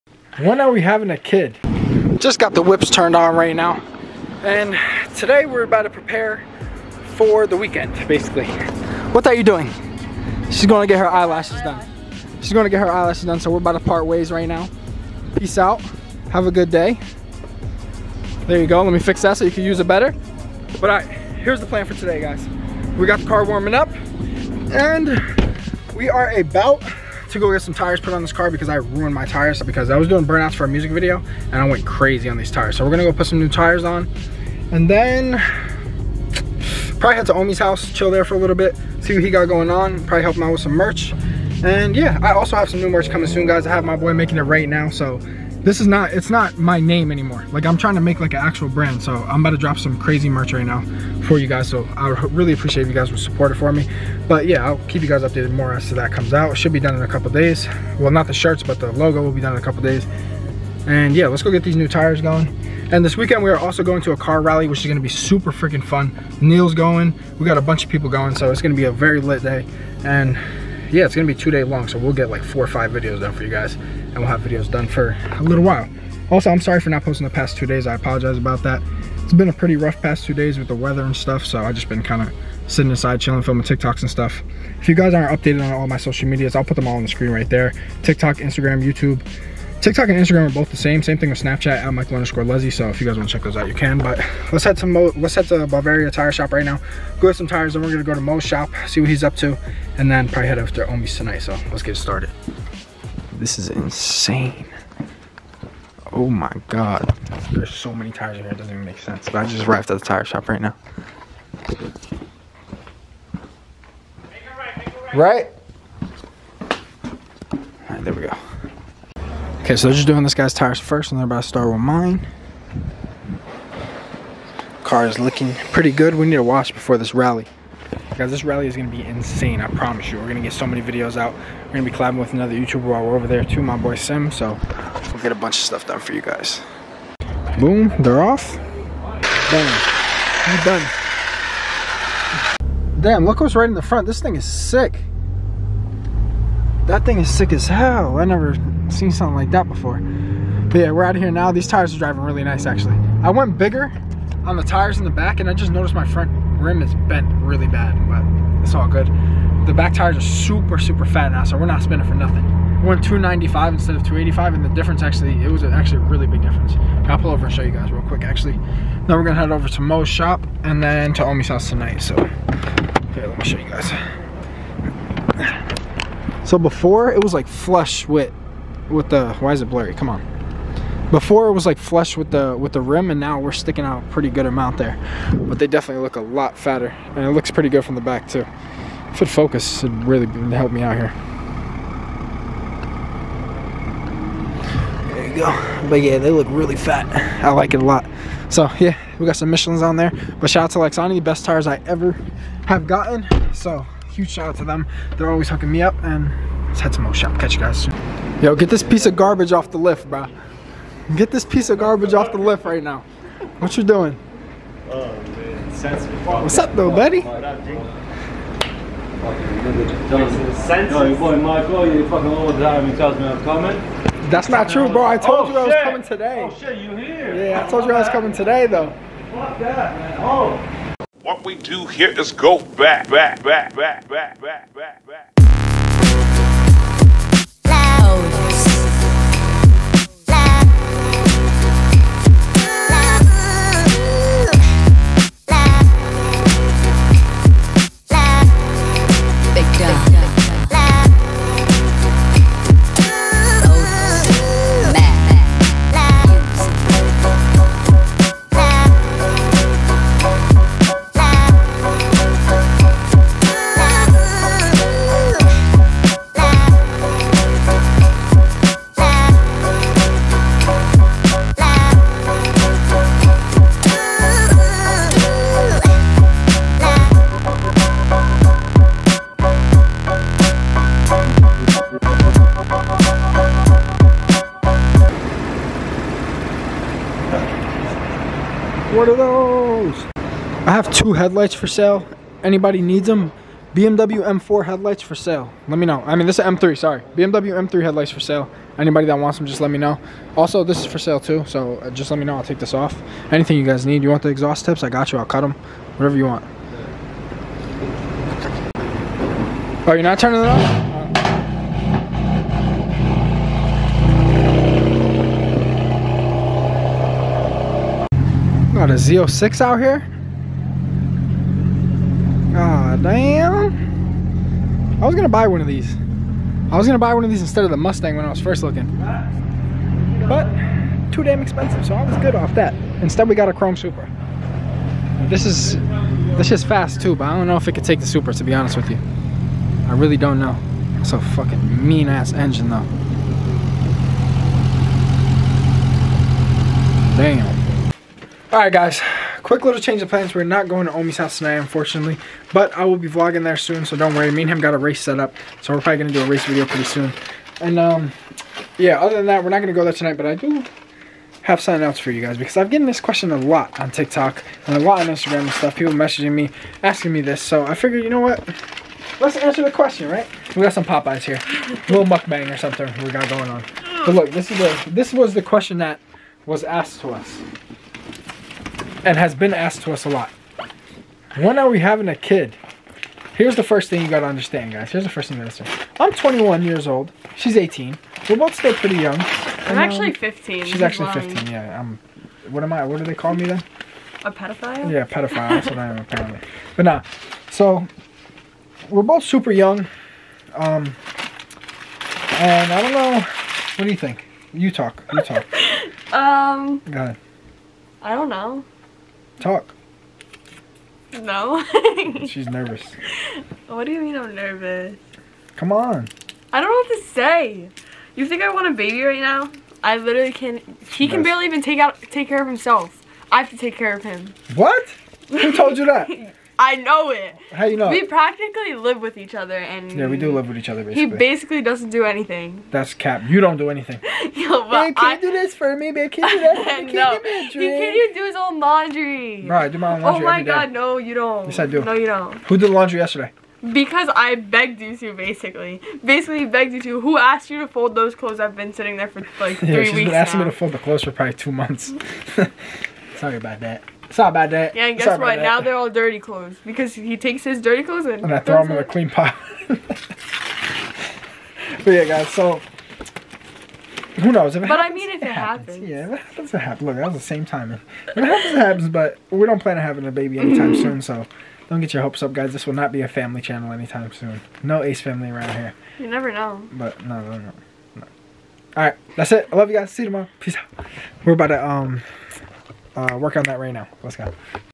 When Are We Having a BABY!? + NEW CAR TIRES
We got this question a lot so here is the answer... When are we having a baby!! Also we got some new tires put on my supra for the rally we're heading to this weeked!
VLOGMAS EP. 7

Hope you enjoy Have a good day everyone!

Looking to Buy a CAR!?! Check out